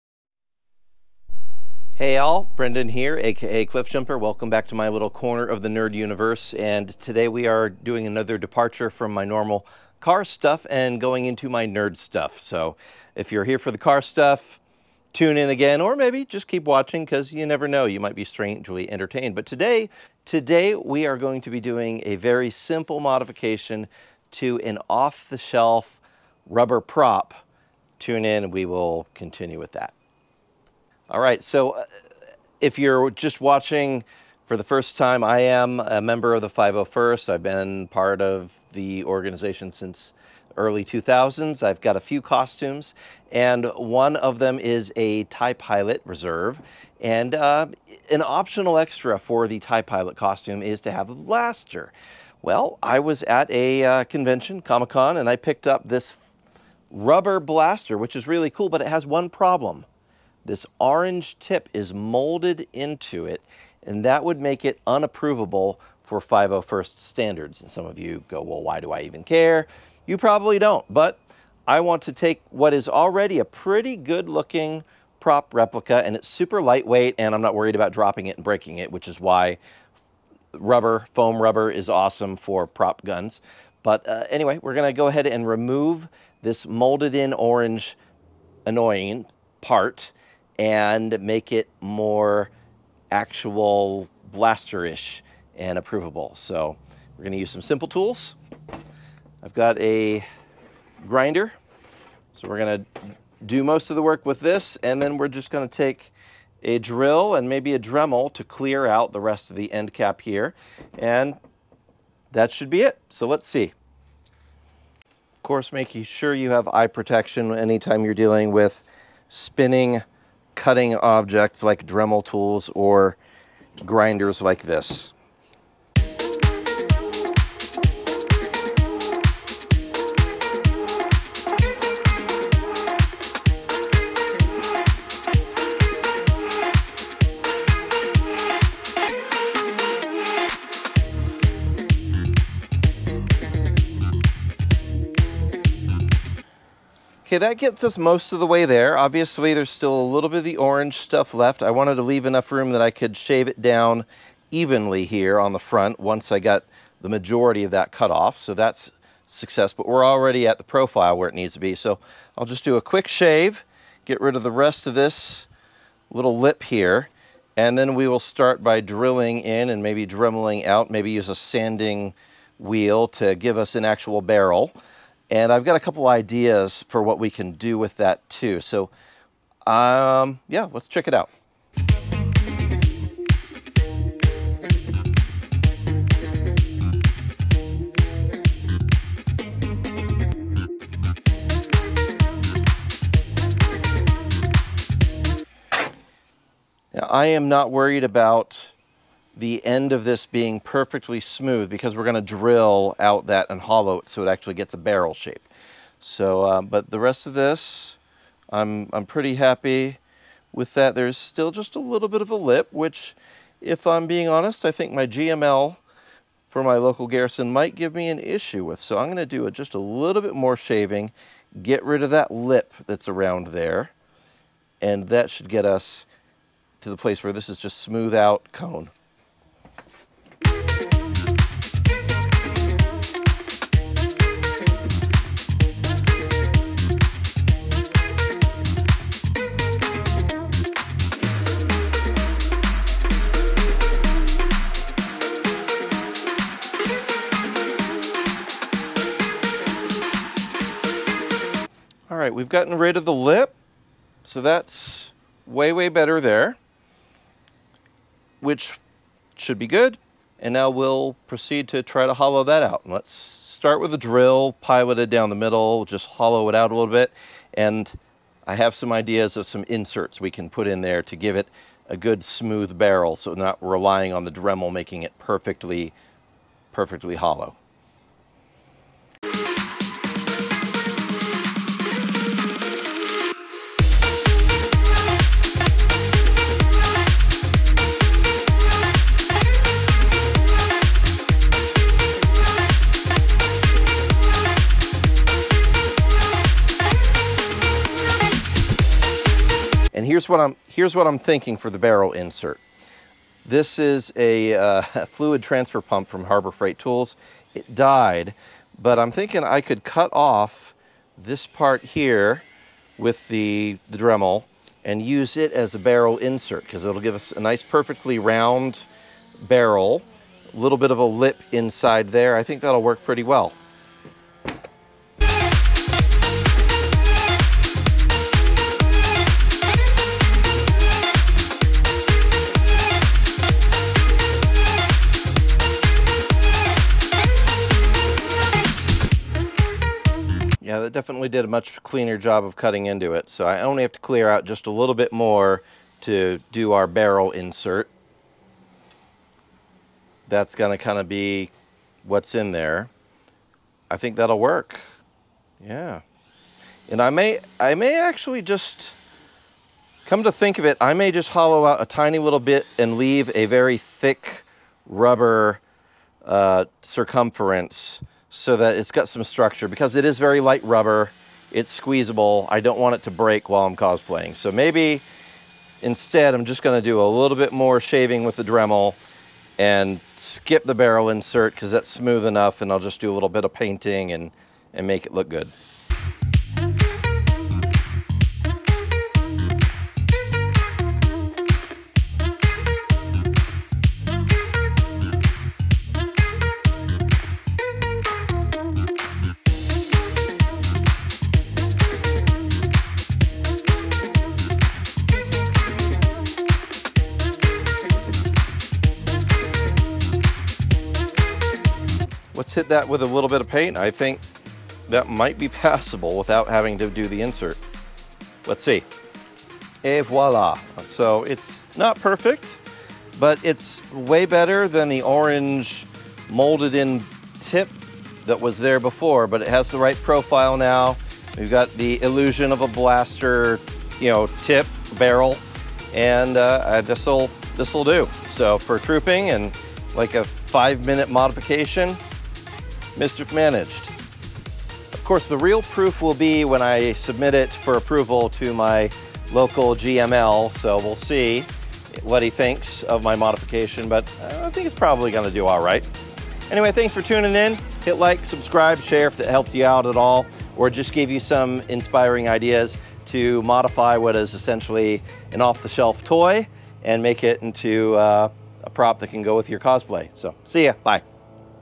Cosplay Tips: Foam Blaster Modification to Remove Orange Cap
This is just a really simple modification you can do to one of those lightweight foam blasters, whether it's a DL-44, a Greedo Killer, an Imperial Officer Merson Power 5 sidearm, or pretty much any foam blaster that has the annoying orange disc molded into it.   The next convention is always just around the corner and you want your gear to look on point!